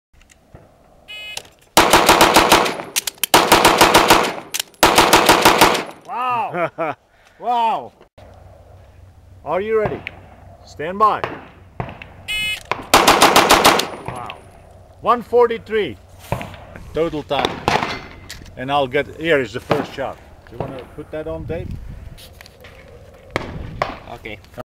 Bill Drill (M2i Custom Open Gun 9mm Major)
Time: 4.59 (18 Shots)
Time: 1.43 (6 Shots)
Gun: M2i Custom Gun by Matt Mclearn